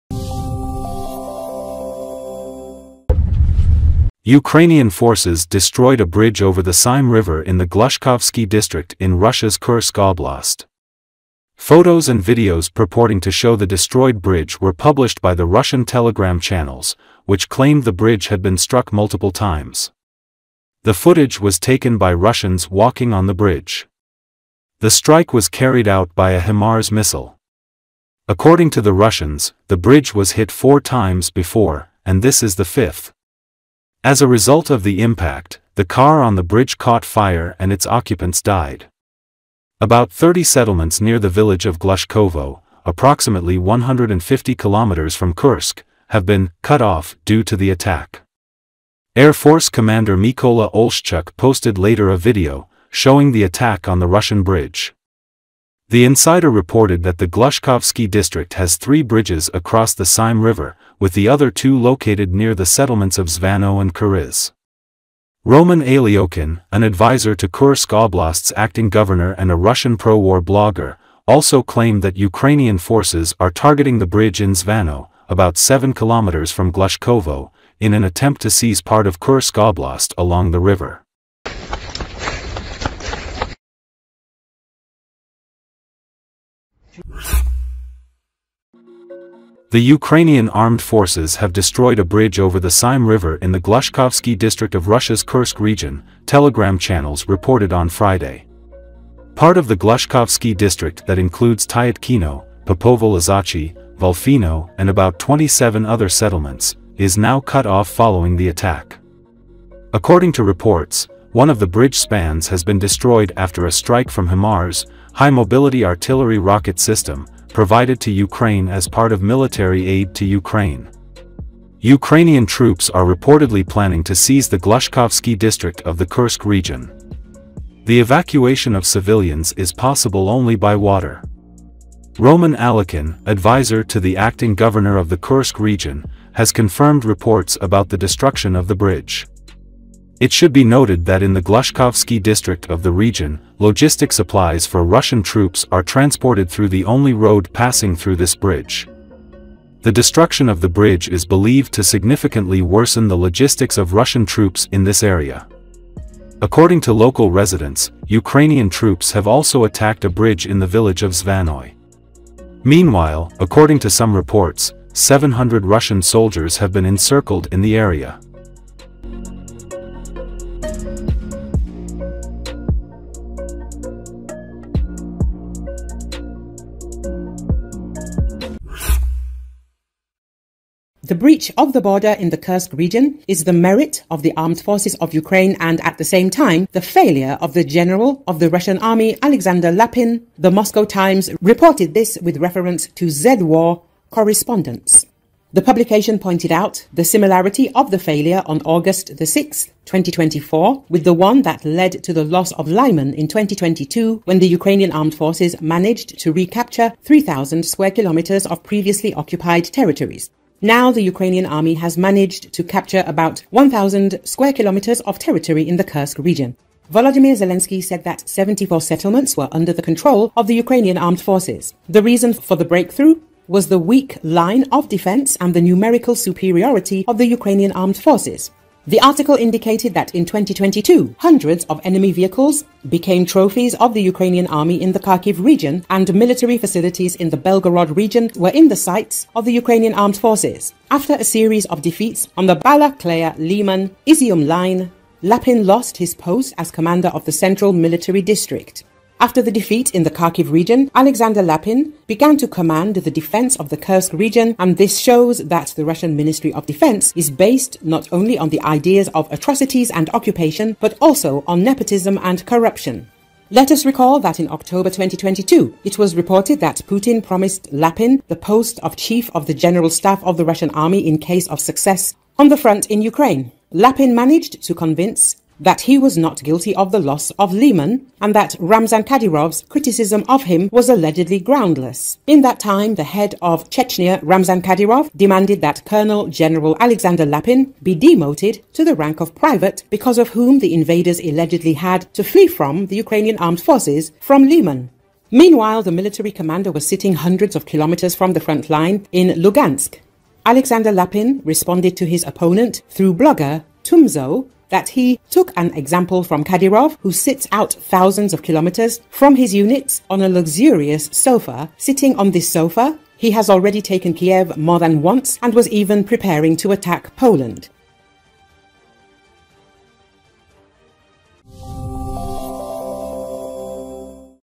İmage of bridge destroyed by Ukrainians with HIMARS in Kursk - it is destroyed, car is on fire...
Photos and videos purporting to show the destroyed bridge were published by the Russian Telegram channels, which claimed the bridge had been struck multiple times. The footage was taken by Russians walking on the bridge. The strike was carried out by a HIMARS missile. According to the Russians, the bridge was hit 4 times before, and this is the fifth. As a result of the impact, the car on the bridge caught fire and its occupants died.

About 30 settlements near the village of Glushkovo, approximately 150 kilometers from Kursk, have been "cut off" due to the attack.

Air Force Commander Mykola Oleshchuk posted later a video, showing the attack on the Russian bridge.

The Insider reported that the Glushkovsky district has three bridges across the Seim River, with the other two located near the settlements of Zvannoe and Karyzh. Roman Alyokhin, an advisor to Kursk Oblast's acting governor and a Russian pro-war blogger, also claimed that Ukrainian forces are targeting the bridge in Zvannoe, about 7 kilometers from Glushkovo, in an attempt to seize part of Kursk Oblast along the river.